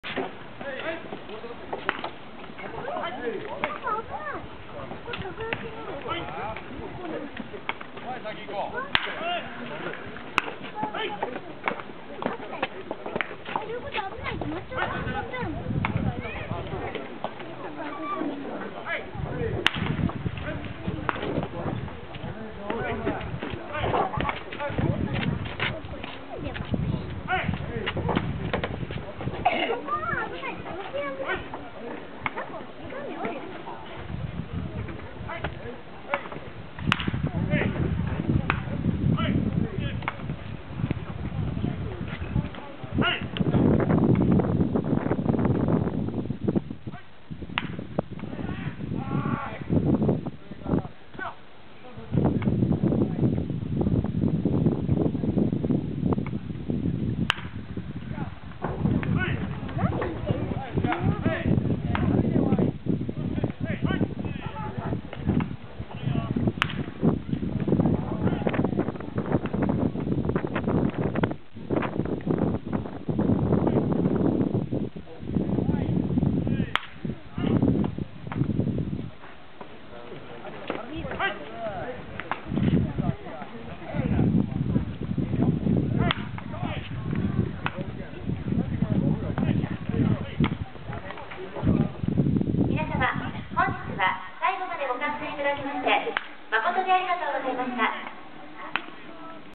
20080309_新井良太に打撃指導する辻2軍監督
ナゴヤ球場でのプロ・アマ交流戦。
中日ドラゴンズvs東海理化。
試合後の練習での新井と辻2軍監督の一面。